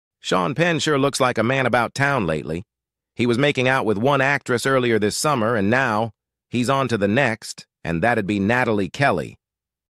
SEAN PENNMAJOR PDA WITH NATHALIE KELLEY
Video/Image Present- From Instagram/google

Sean Penn sure looks like a man about town lately -- he was making out with one actress earlier this summer, and now ... he's on to the next, and that'd be Nathalie Kelley.